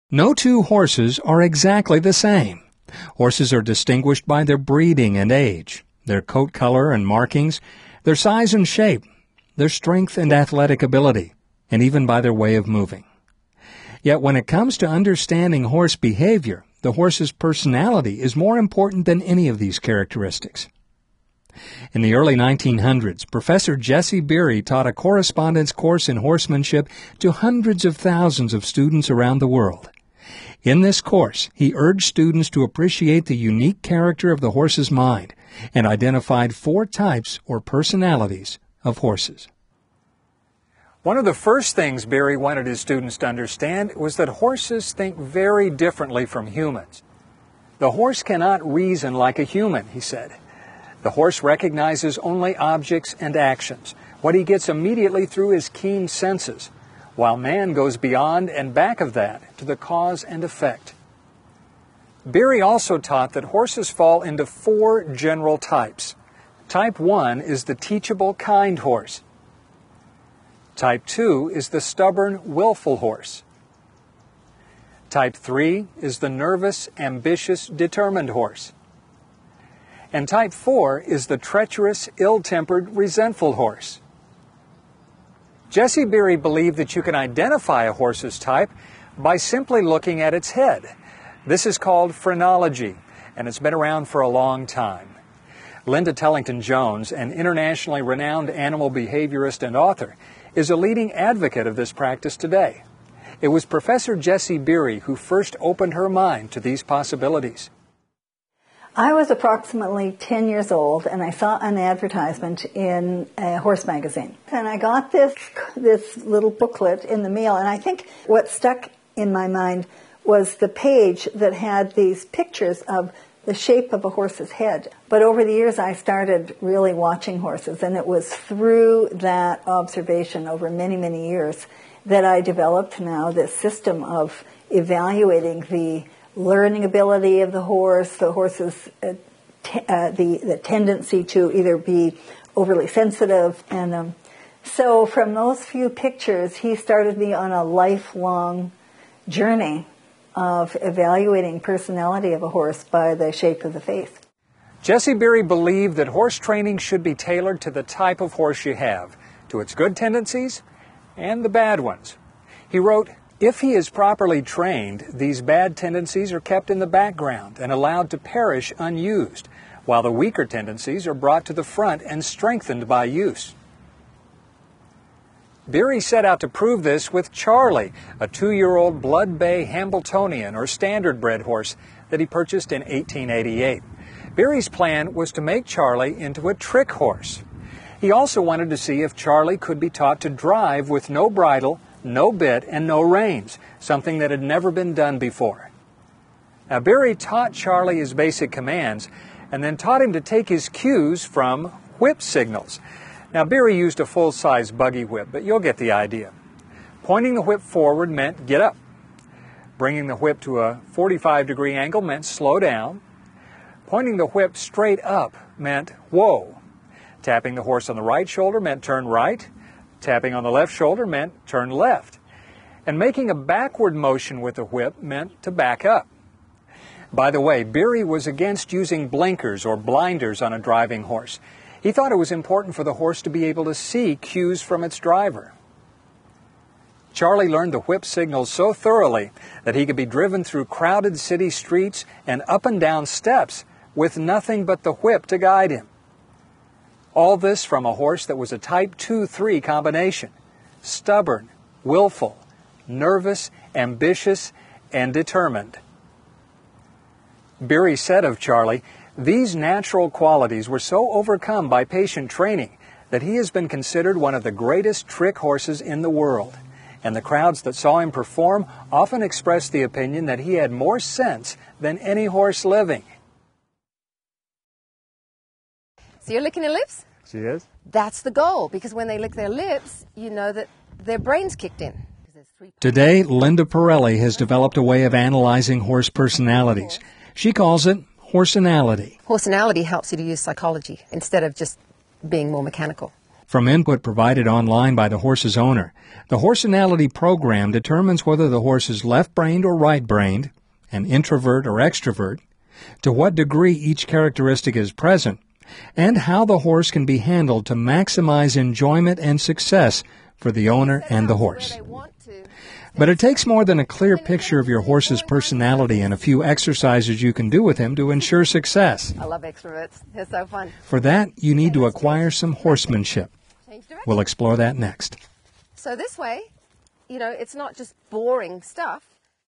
Horse Personalities - by Rick Lamb
This is a short video I did to explain the concept of horse personalities. It features the work of Professor Jesse Beery, Linda Tellington-Jones, and Linda Parelli. Enjoy!